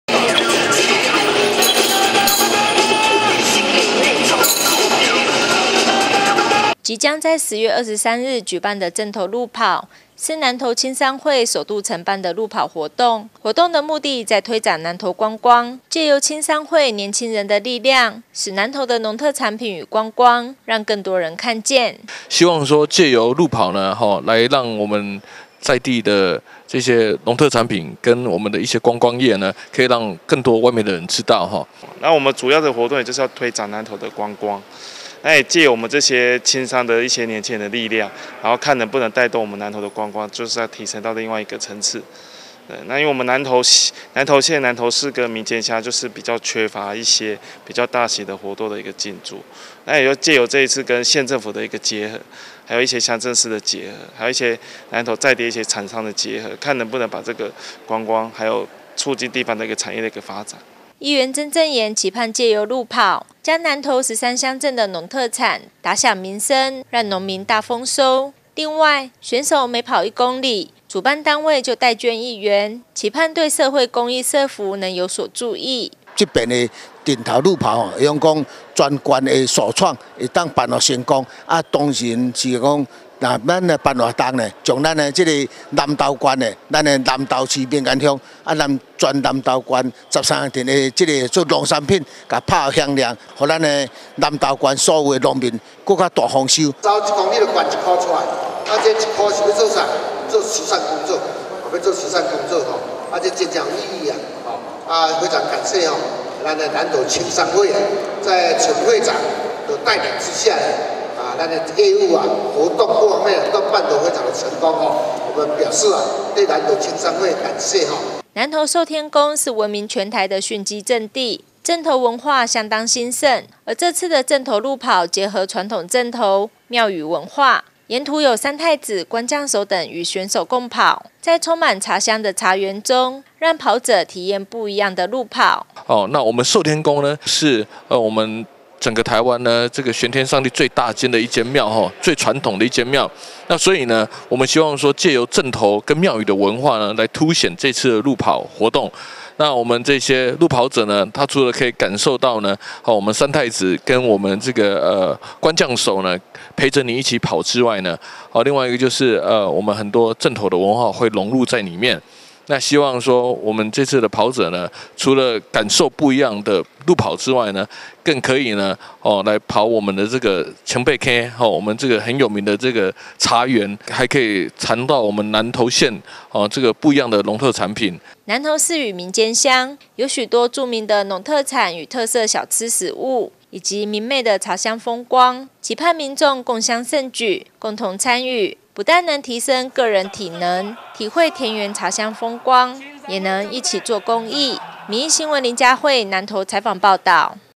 1050803 民議新聞 就是愛鬥陣RUN南投 全台首創陣頭路跑(議員 陳翰立 曾振炎)
為促進南投地方產業，與文化及觀光發展，南投縣政府與受天宮主辦「南投國際青商會」路跑活動，並首次結合陣頭及公益，而青商會長同時也是議員陳翰立，歡迎民眾一起為健康而跑。